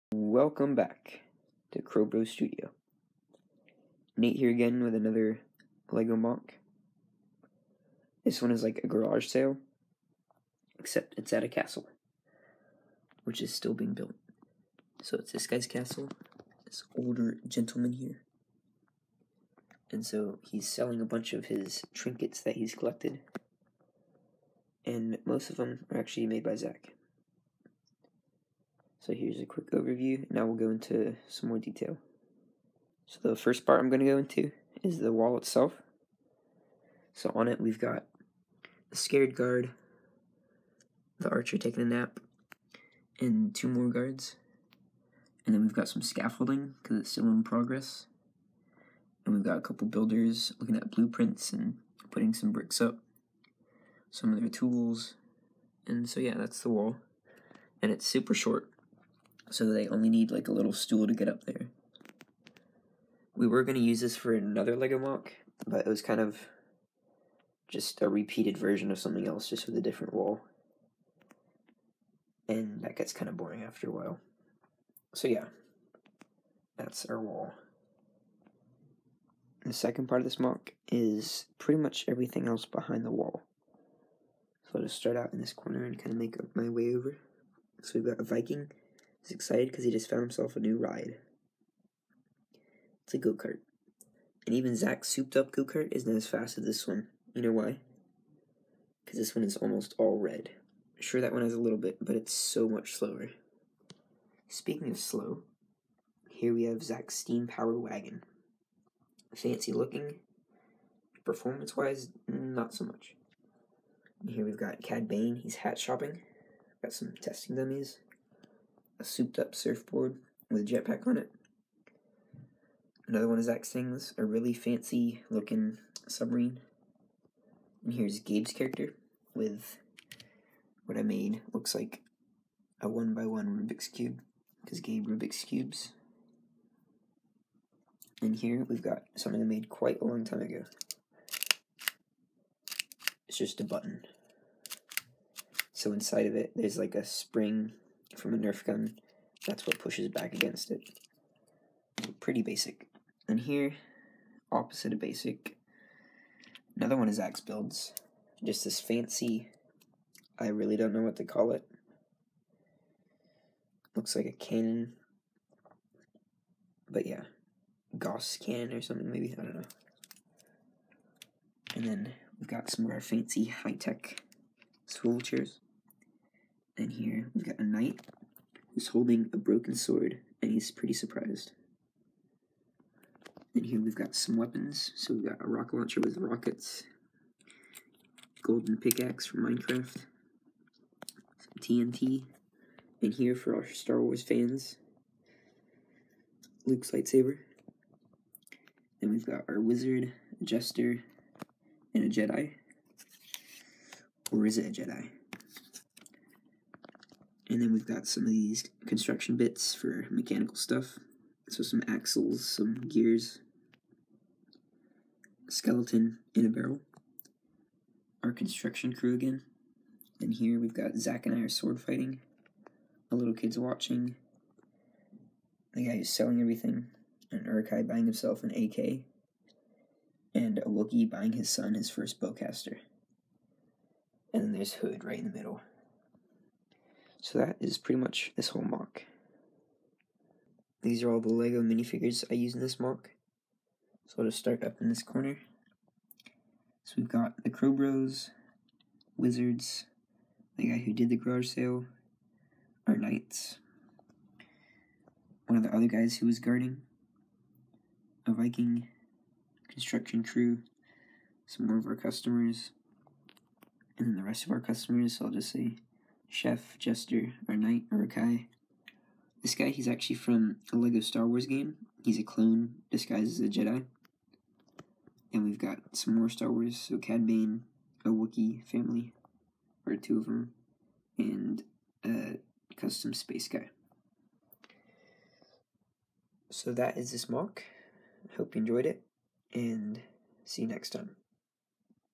LEGO Garage Sale MOC
Yet another one of N8’s creations, along with some of Zac's cool builds.

This video was put together using the Splice Video Editor App.